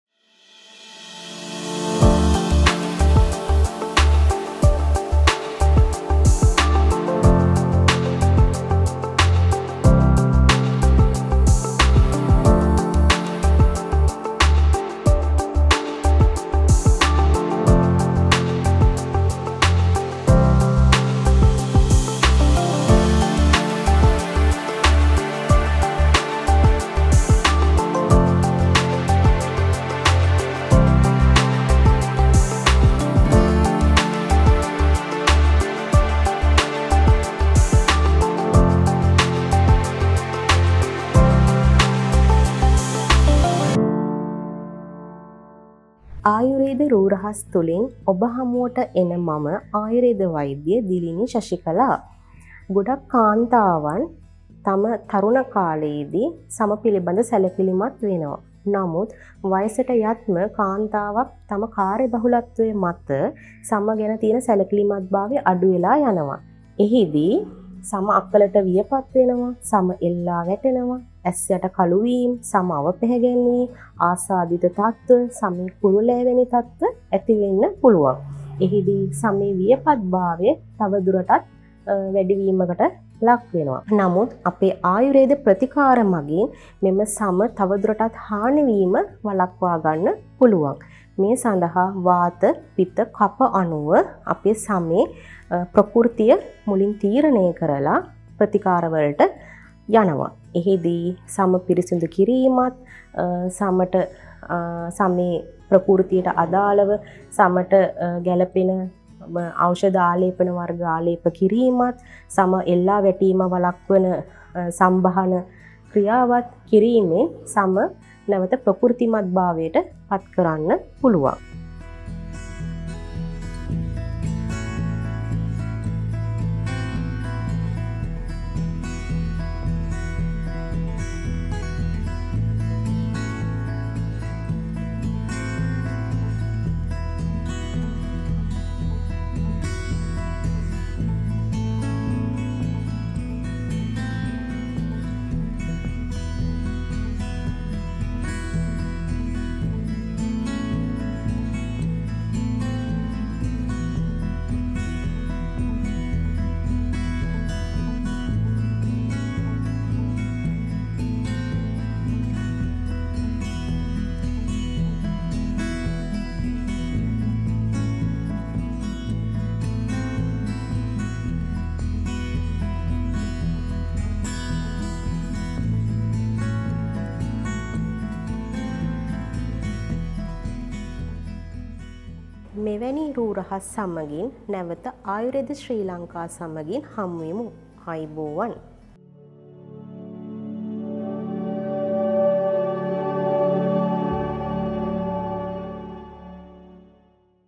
වයස හංග ගෙන තරුණ වෙන්නෙ මෙහෙමයි | Ayurveda Sri Lanka
ආයුර්වේද ශ්‍රී ලංකා තුළින් අද අපේ කතා බහට ලක්වන්නේ සමේ වියපත් බව නැති කරන ආයුර්වේදීය ප්‍රතිකාර ක්‍රම පිළිබඳවයි. මේ සම්බන්ධව ඔබව දැනුවත් කරන්නට එකතු කරගත්තේ ආයුර්වේද වෛද්‍ය  දිලිනි ශෂිකලා මහත්මියවයි. මේ පිළිබඳව හරියටම දැනගන්න රැදෙන්න දිගටම Ayurveda Sri lanka සමඟින්..